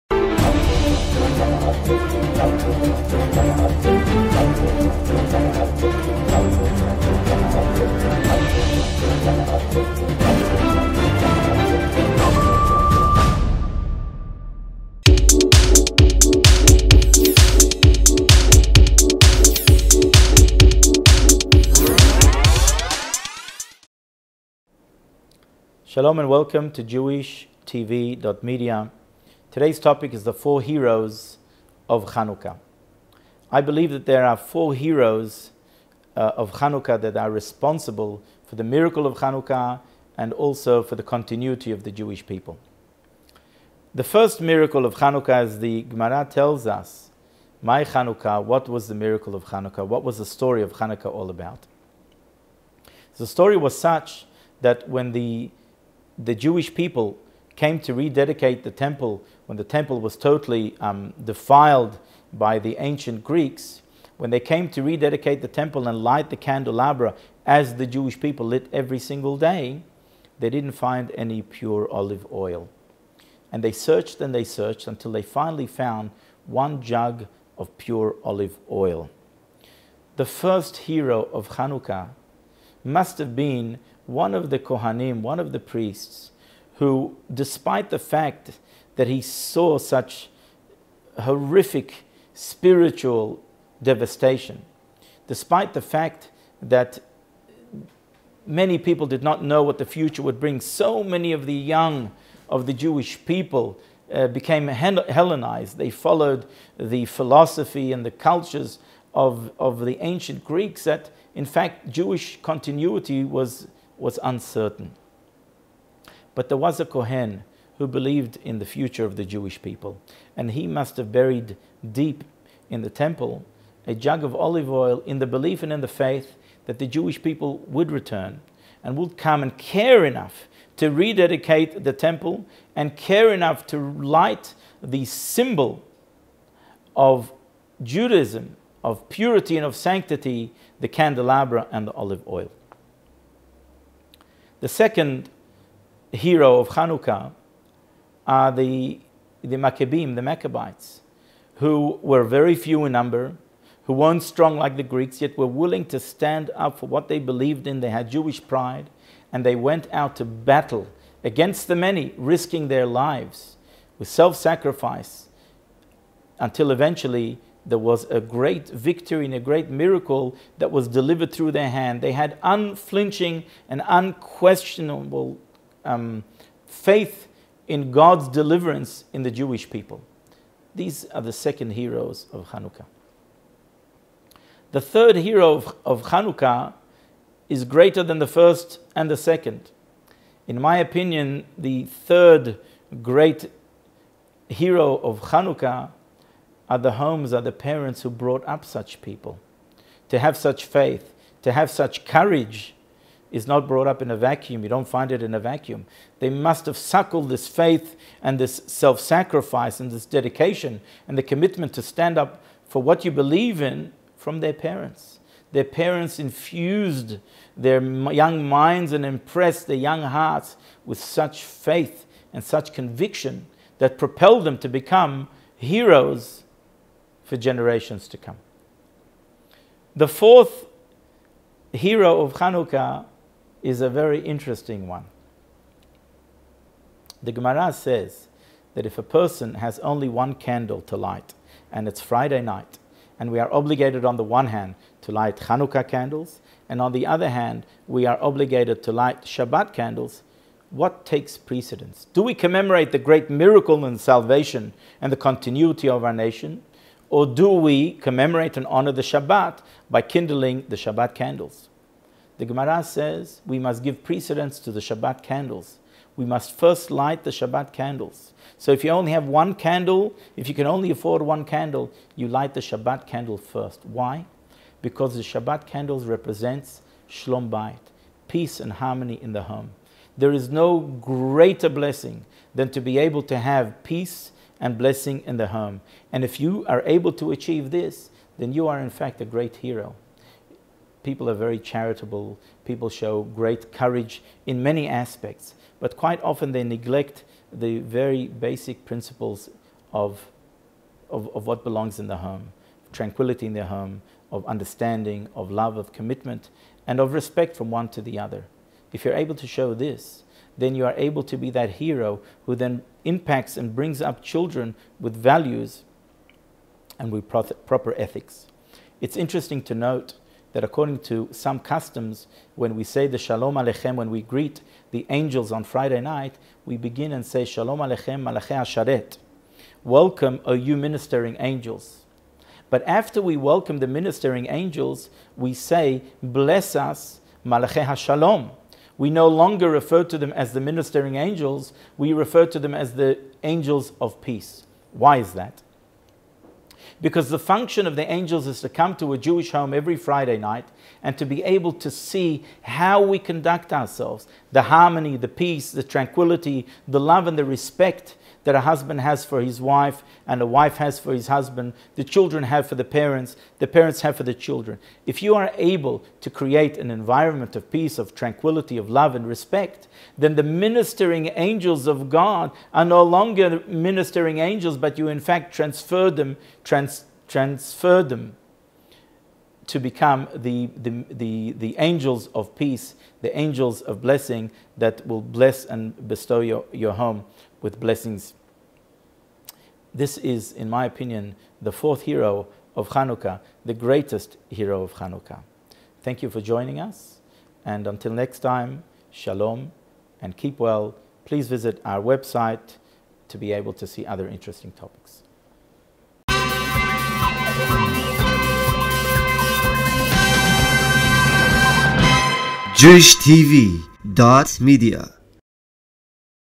The 4 Heroes Of Chanukah
Rabbi Michael Chriqui, Rabbi of The Sephardi Synagogue in Sydney Australia, in a fascinating series of lectures in Jewish topics.